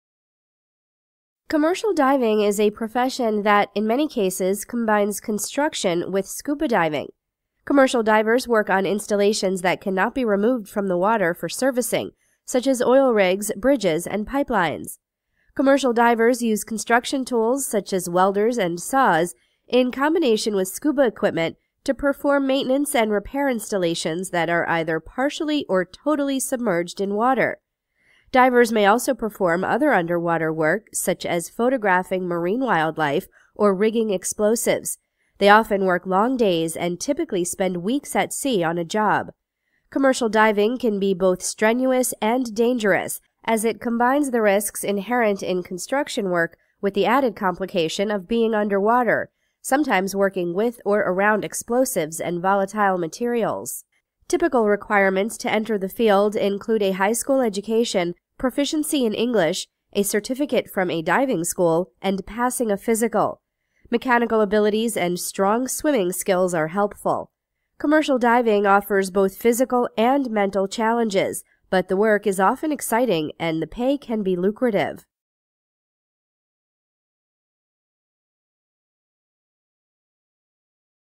Commercial Diver Career Video
JOB TITLE: Commercial Divers

OCCUPATION DESCRIPTION: Work below surface of water, using surface-supplied air or scuba equipment to inspect, repair, remove, or install equipment and structures. May use a variety of power and hand tools, such as drills, sledgehammers, torches, and welding equipment. May conduct tests or experiments, rig explosives, or photograph structures or marine life.

RELATED JOB TITLES: Commercial Diver, Dive Superintendent, Dive Supervisor, Diver, Diver Tender, Diving Supervisor, Hard Hat Diver, Non Destructive Testing Under Water Welder (NDT U/W Welder), Salvage Diver, Tender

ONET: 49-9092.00